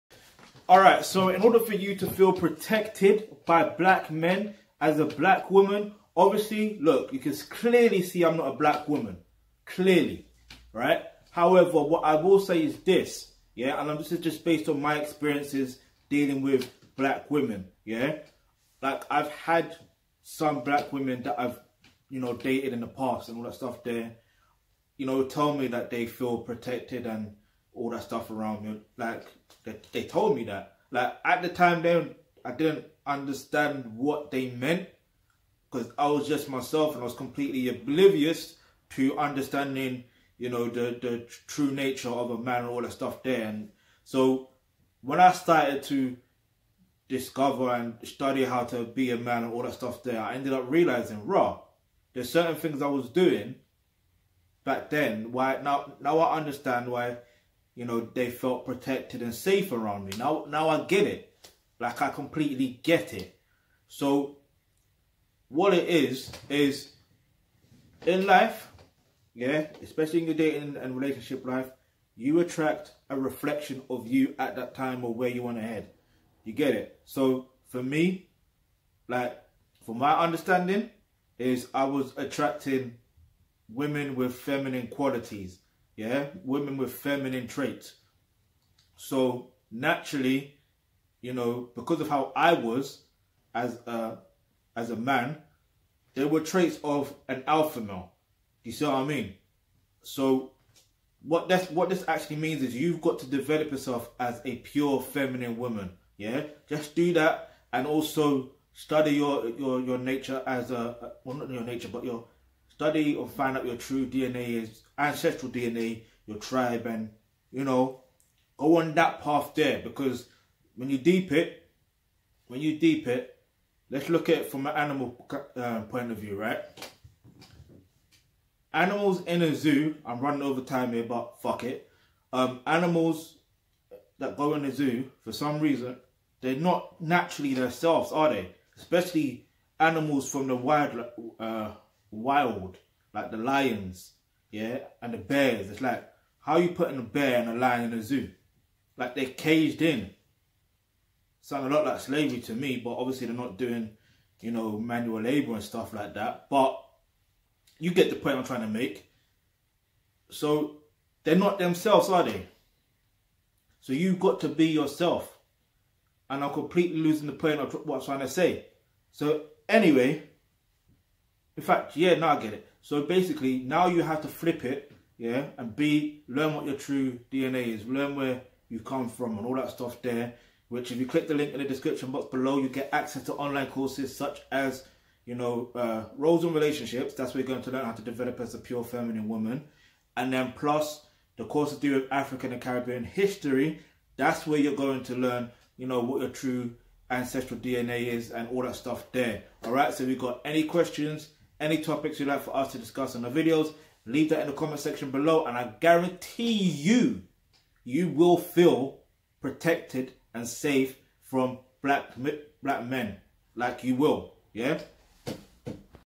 How Black Women Can Feel Protected by Black Men
On the RoadTo1Billion Subscribers...in this video RaphaelOnochie aka Versatile Onochie-Obayuwana explains how BlackWomen Can Feel Protected by BlackMen...